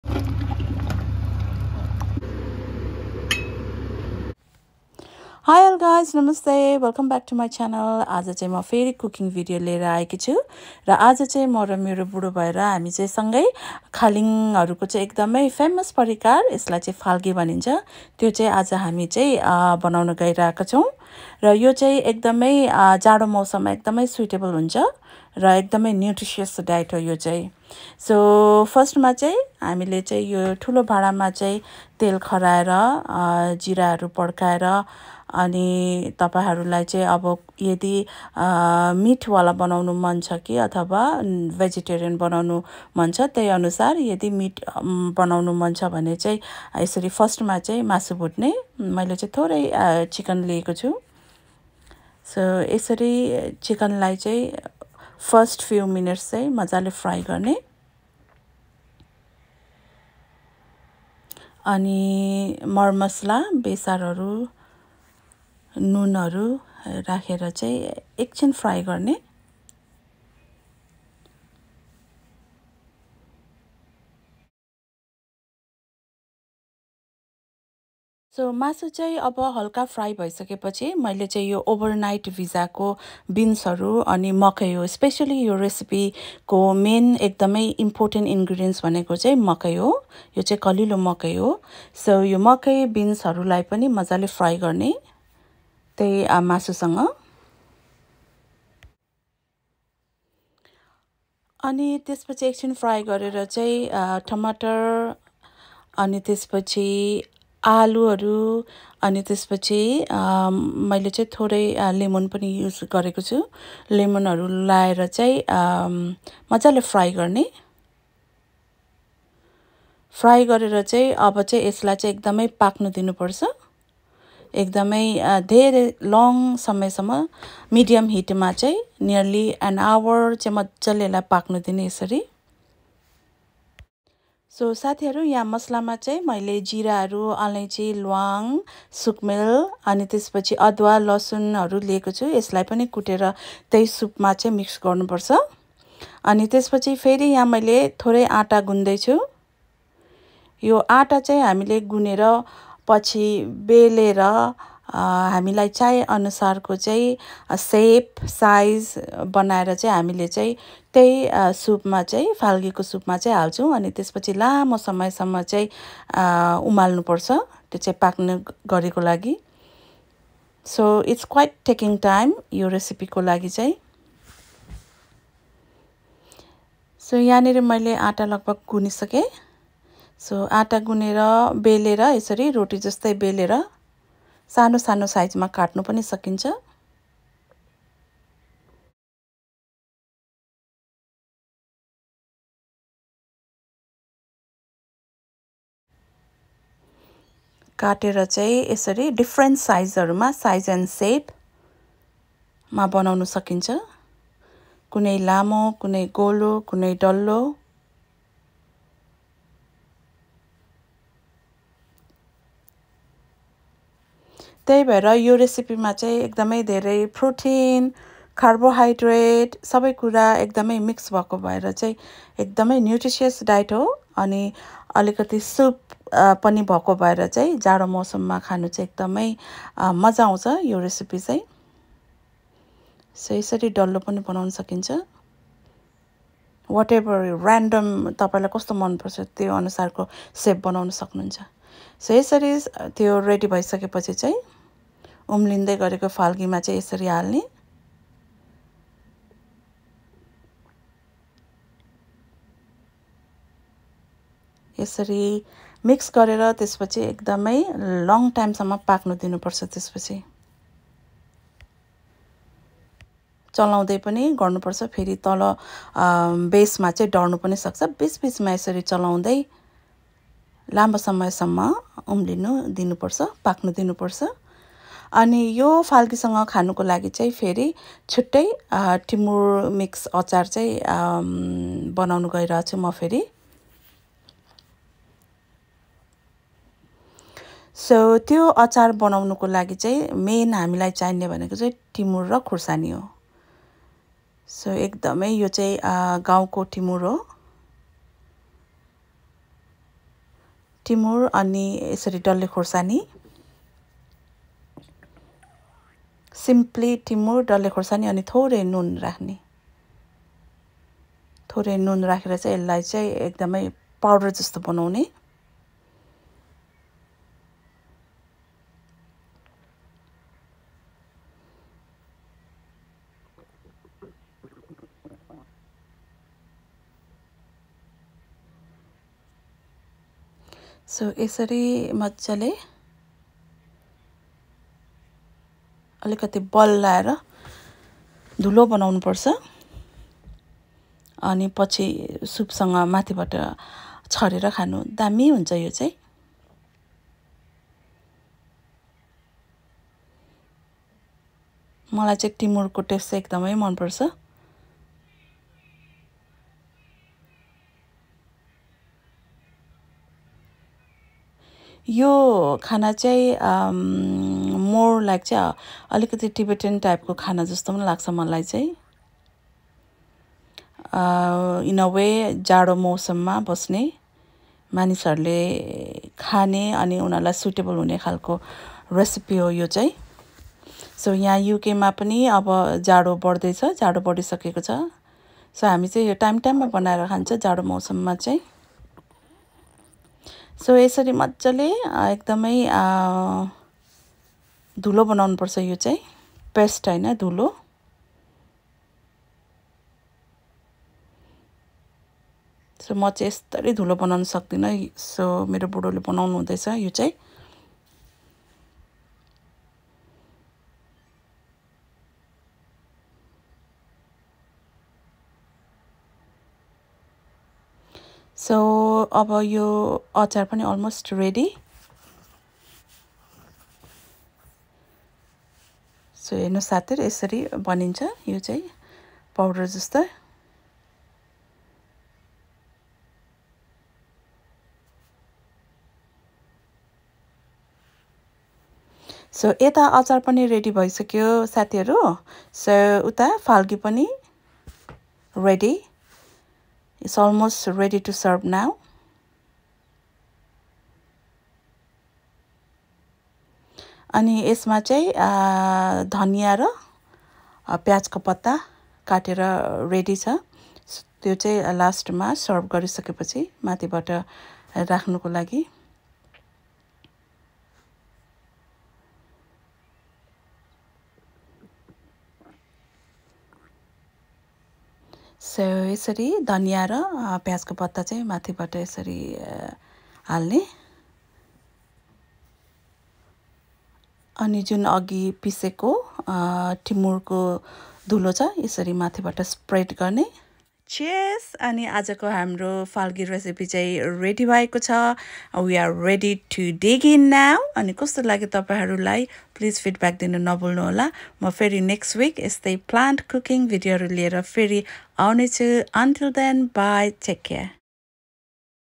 Falgi recipe | Highly nutritious authentic nepali food
Falgi is very nutritious diet with mixture of vegetables,protein, carbohydrates,minerals famous in himalayan areas of Nepal specially in  Solukhumbu among khaling and Sherpa communities!!